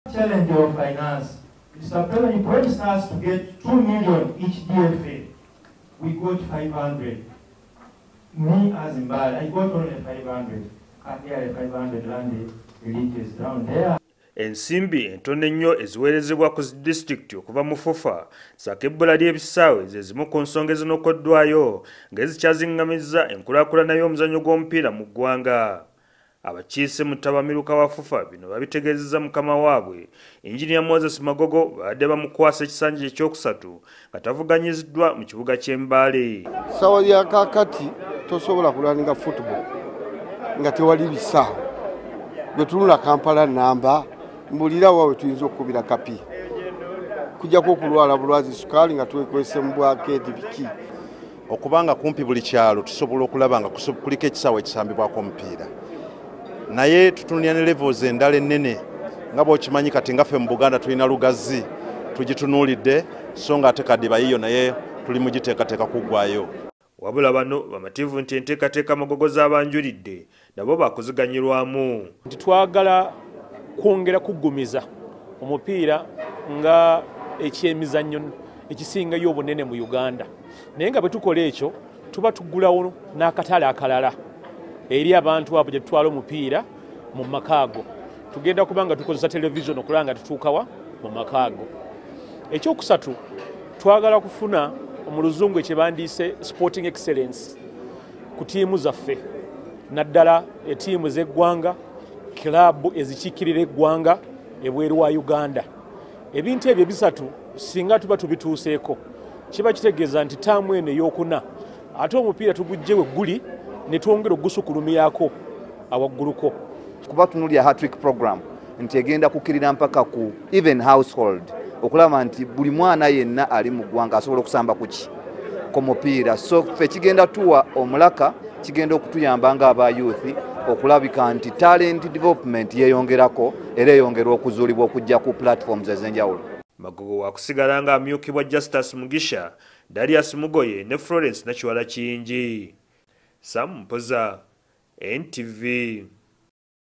Ebbula ly’ebisaawe ligotaanyizza okukulaakulanya omupiira
Abakiise mu ttabamiruka wa FUFA eyatudde mu kibuga Mbale bagamba nti ebbula ly'ebisaawe mu bitundu byebakiikirira wamu n'ensimbi entono zebafuna okuva mu FUFA okuddukanya omupiira mu bitundu byabwe bigotanyiza entekateeka ey'okutumbula omupiira mu ggwanga.